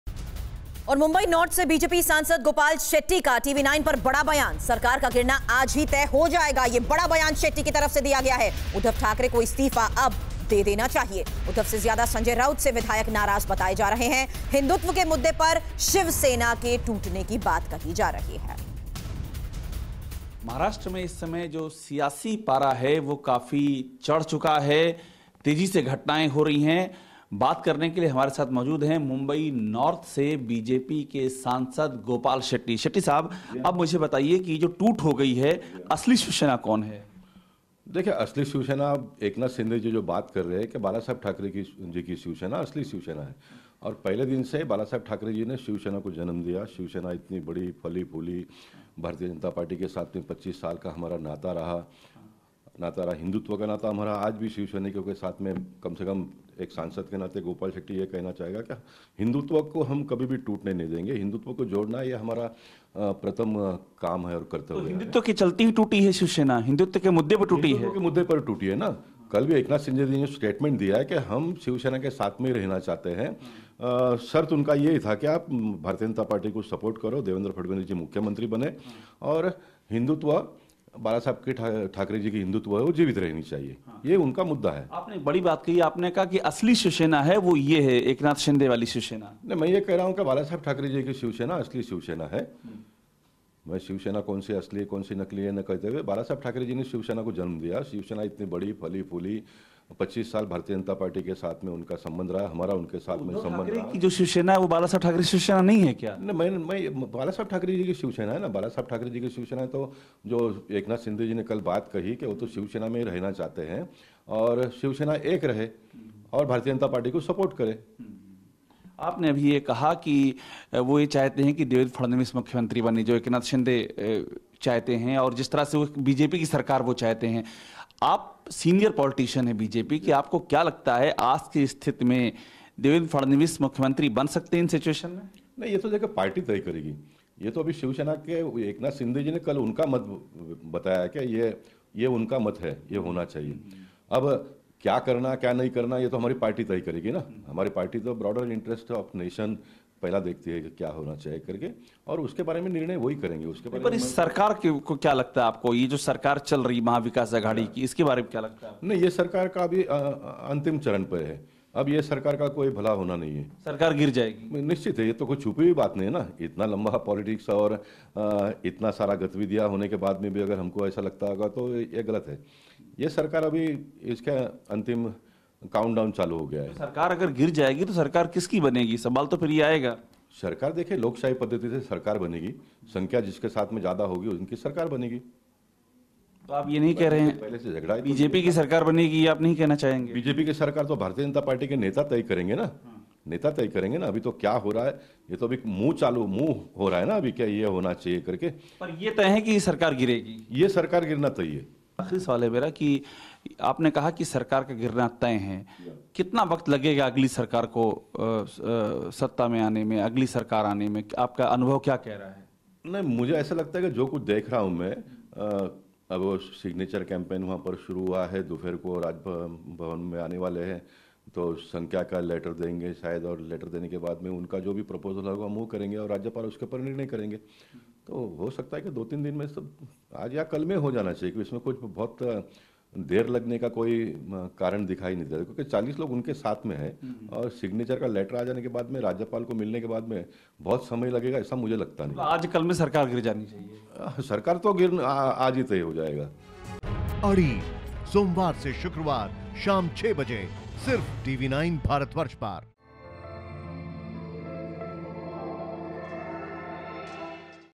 Maharashtra Crisis: BJP सांसद Gopal Shetty का बड़ा बयान, Uddhav Thackeray को इस्तीफा दे देना चाहिए
मुंबई नॉर्थ से BJP सांसद गोपाल शेट्टी का TV9 पर बड़ा बयान, कहा- सरकार का गिरना आज ही तय हो जाएगा,उद्धव ठाकरे को इस्तीफा दे देना चाहिए,हिंदुत्व के मुद्दे पर टूट रही है शिवसेना.

Credit